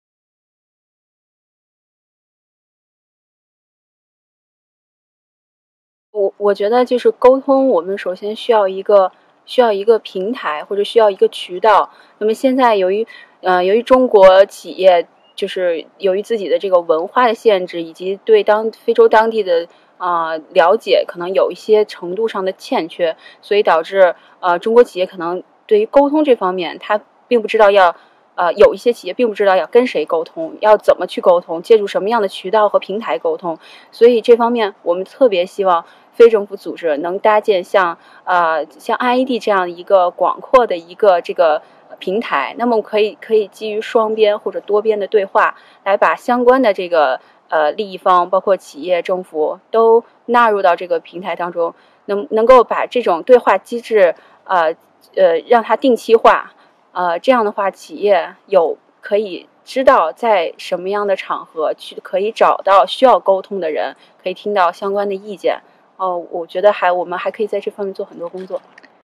China-Africa Forest Governance Learning Platform: Li Ming interview
Li Ming, associate researcher at the Chinese Academy of Forestry, attended her first platform event in Beijing last October 2016. For her, the platform plays a critical role in providing a channel of communication for Chinese companies operating in Africa to interact with NGOs and government.

Ming was speaking at the third learning event of the China-Africa Forest Governance Learning Platform, which brought together more than 100 participants in Beijing to discuss how China can help sustain Africa's forest resources

The event took place in Beijing from 24-25 October, 2016.